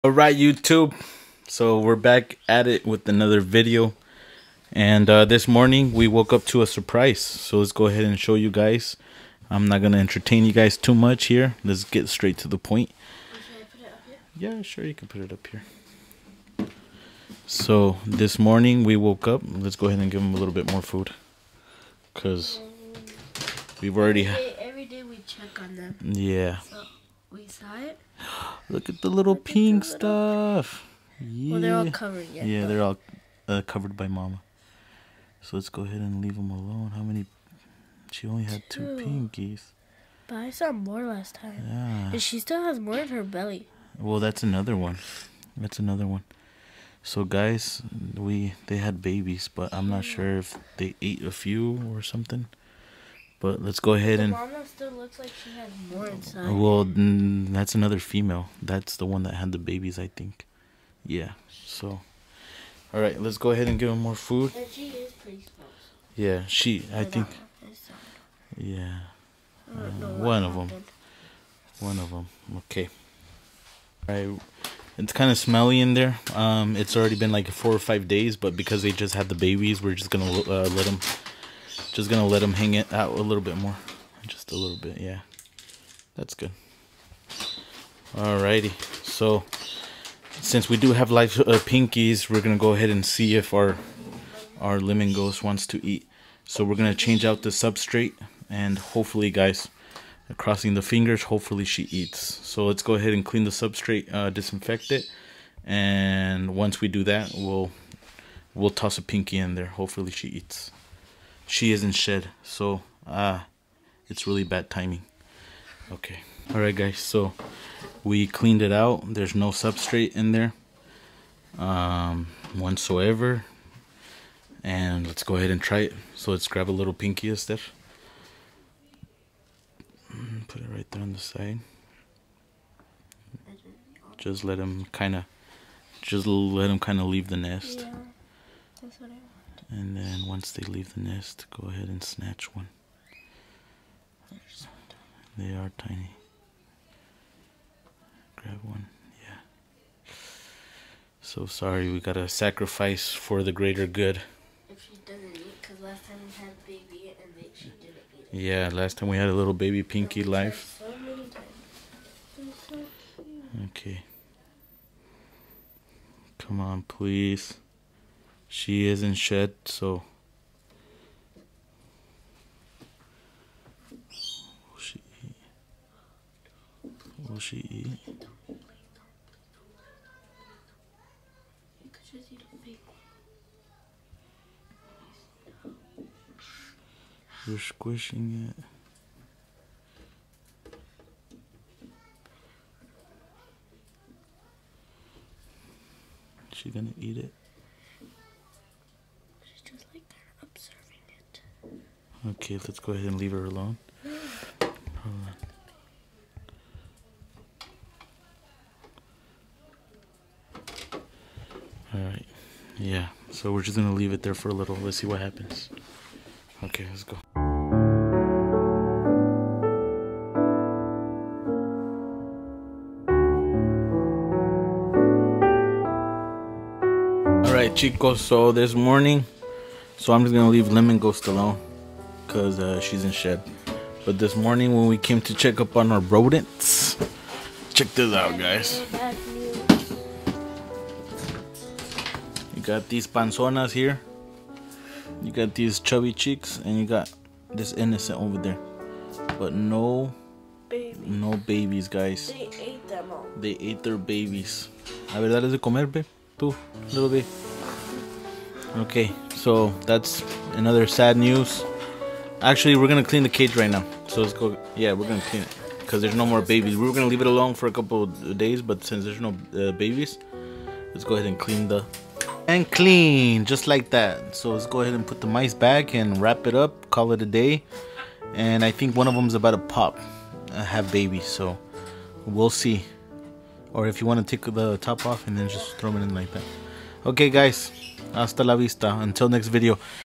Our cannibalistic mice “gave” us babies for our hognose
Our mice have suddenly moved to the center spotlight on stage. we have high expectations for them, will our LG be able to eat live pinkies? what happened to the pinkies? questions answered on this hognose mice breeder series. thank you and enjoy the video.